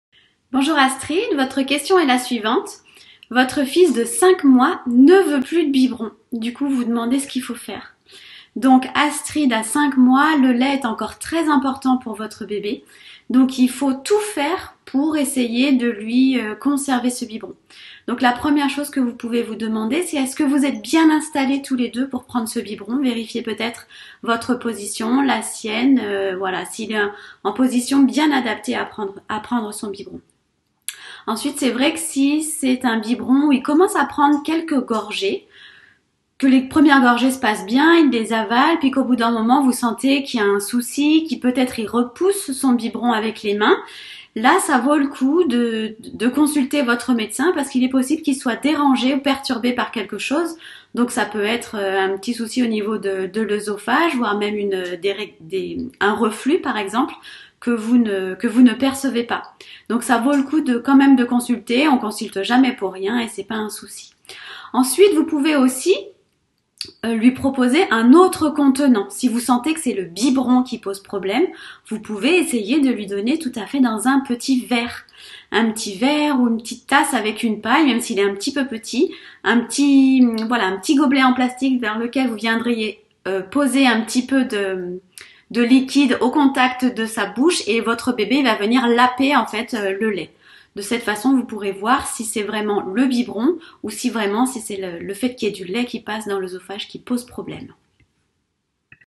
Que faire quand mon bébé de 5 mois ne veut plus de biberon ?
Question :

Bébé de 5 mois ne veut plus de biberons que faire cela devient compliqué

Réponse :

À 5 mois, le lait est encore très important pour votre bébé. Il faut tout faire pour essayer de conserver ce biberon. La première chose que vous pouvez vous demander c’est : est-ce que vous êtes bien installés tous les deux pour prendre ce biberon ? Vérifiez peut-être votre position, la sienne, s’il est bien en position bien adaptée à prendre son biberon...

Vous trouverez en plus de cela d'autres conseils sur l'alimentation de votre bébé ainsi que de nombreuses recettes sur le site internet !